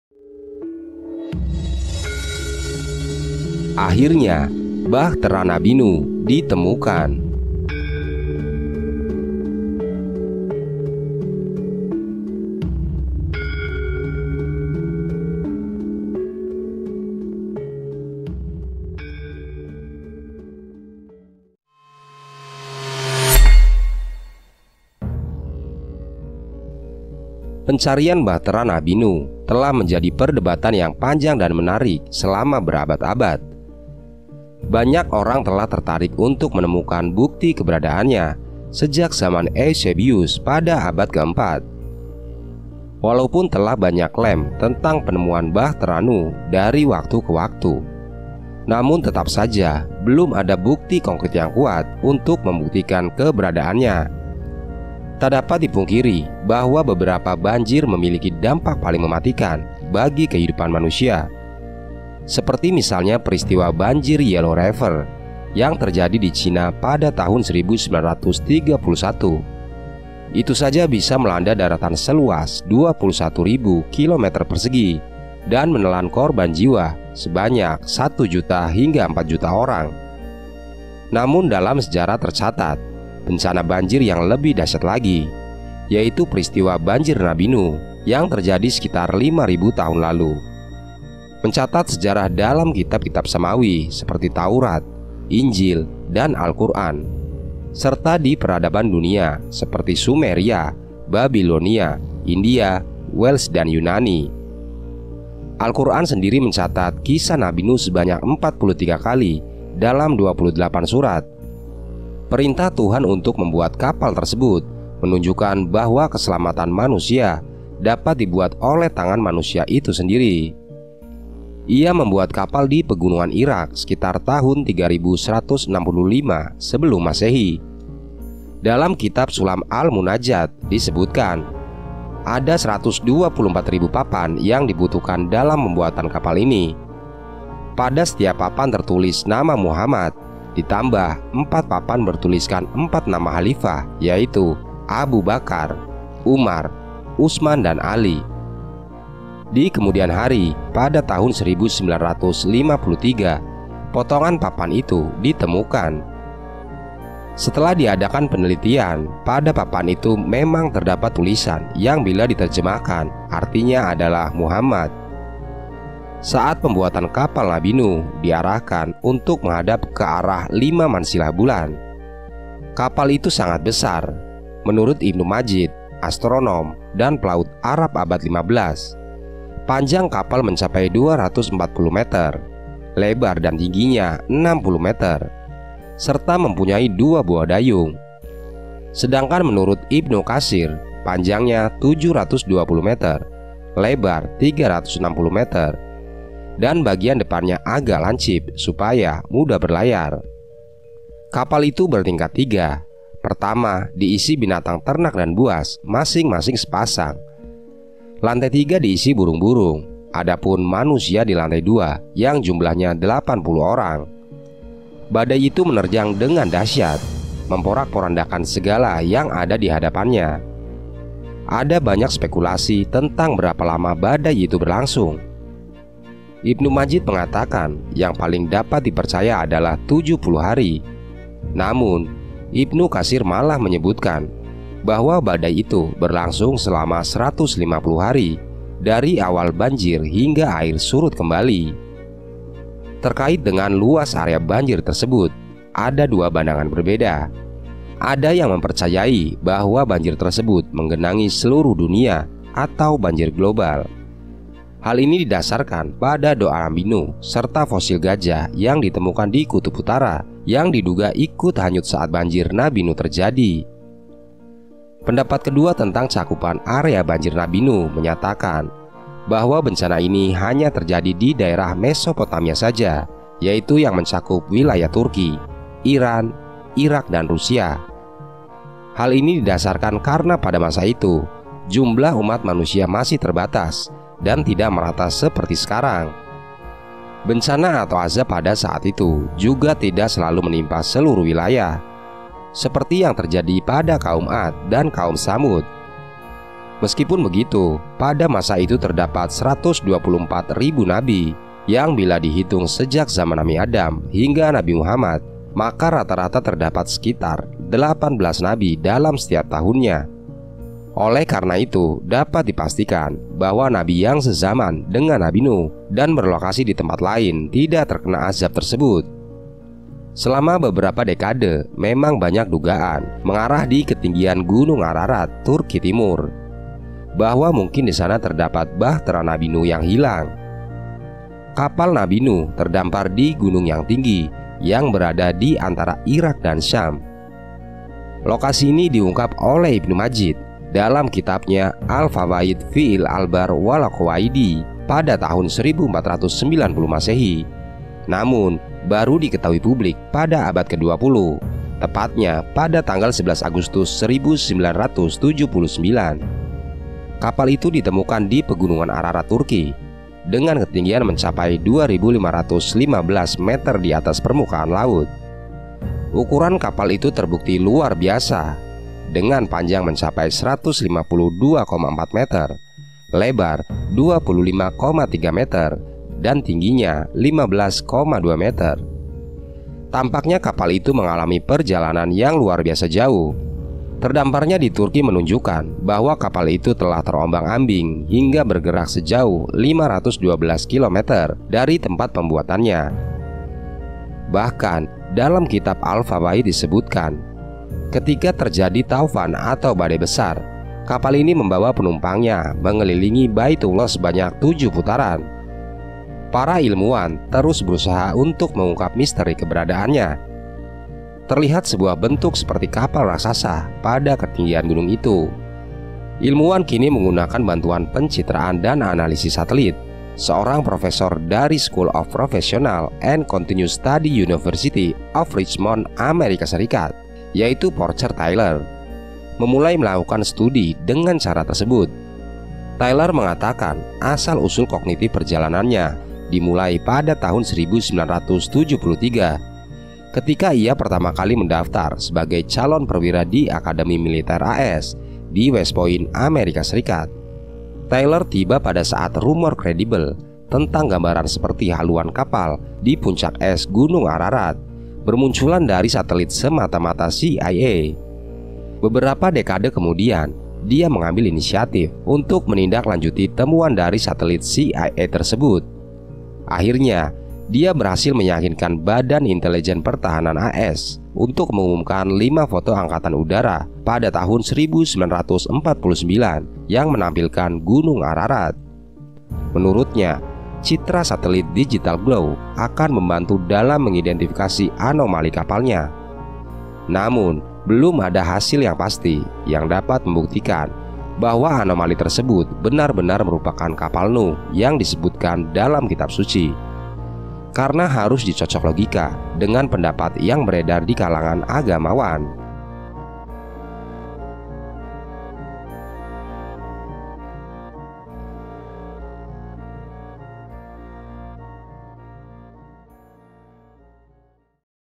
TERNYATA DISINI ❗️❗️ UMAT ISLAM DI DUNIA GEGER ❗️❗️ AKHIRNYA BAHTERA NABI NUH DITEMUKAN
pencarian Bahtera Nabi Nuh telah menjadi perdebatan yang panjang dan menarik selama berabad-abad.
banyak orang telah tertarik untuk menemukan bukti keberadaannya sejak abad ke-4.
walaupun telah banyak klaim tentang penemuan Bahtera Nuh dari waktu ke waktu namun tetap saja belum ada bukti konkrit yang kuat untuk membuktikan keberadaannya.

Temuan-temuan Kapal Nabi Nuh. Sudah sejak lama, pencarian bahtera Nuh menjadi isu yang memicu perdebatan panjang di kalangan para peneliti dan sejarawan. Sejak zaman Eusebius pada abad ke-4, upaya untuk menemukan bukti keberadaan bahtera tersebut terus bergulir hingga pada akhirnya muncul berbagai penemuan menarik, termasuk temuan di gunung Ararat di Turki dan replika yang dibuat berdasarkan tablet tanah liat sumeria. Namun, di balik semua itu, ada pertanyaan yang mendasar, apakah bahtera Nuh benar-benar pernah ada? Dalam narasi kali ini, mari kita telusuri fakta dan temuan menarik seputar pencarian bahtera Nuh, dan mari kita bersama-sama mencari jawabannya.
Music